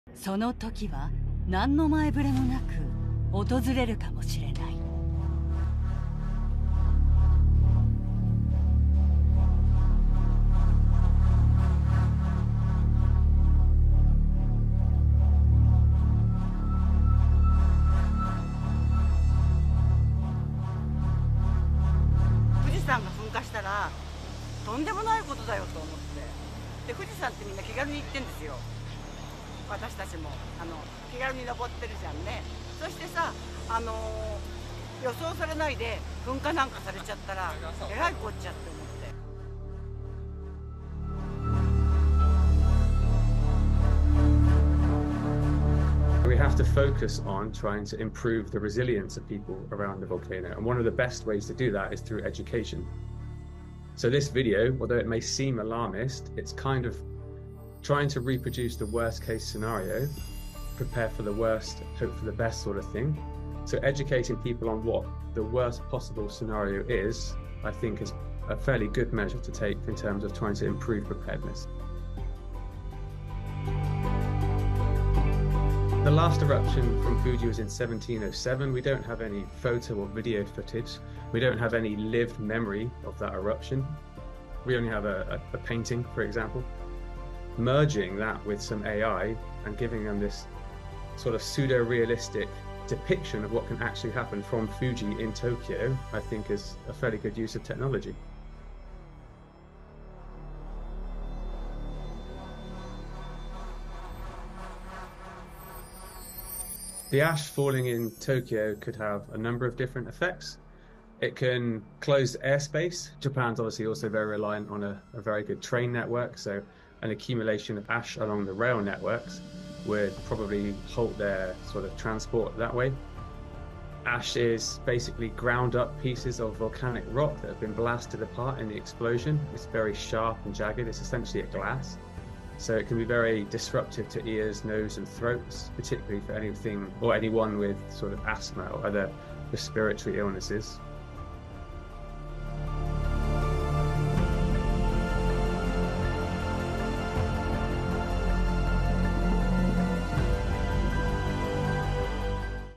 Tokyo uses AI to warn residents to prepare for the worst with simulation of Mount Fuji erupting
The AI-generated video released by Japanese officials for Volcanic Disaster Preparedness Day in Japan shows a simulation of a potential violent eruption of the active volcano.